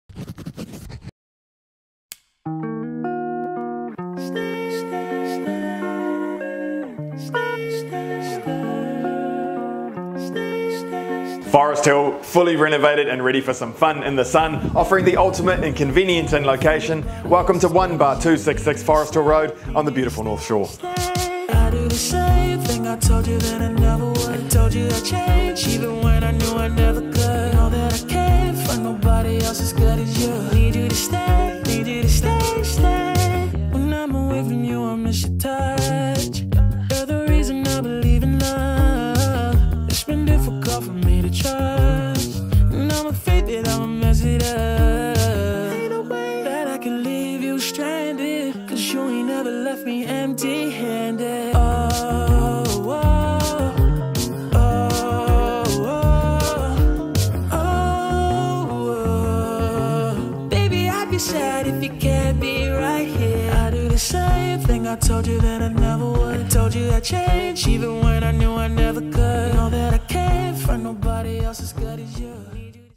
SOLD!! Westlake Weatherboard Winner
Fully renovated and ready for family fun in the sun, this gorgeous weatherboard home ticks all the boxes, in a centralised and uber convenient location.

1/266 Forrest Hill Road, Forrest Hill
3 Bed, 2 Bath, 1 Living, Garage
Campbells Bay School, Sunnynook School, Dual Westlake
harcourts.co.nz/HSF2274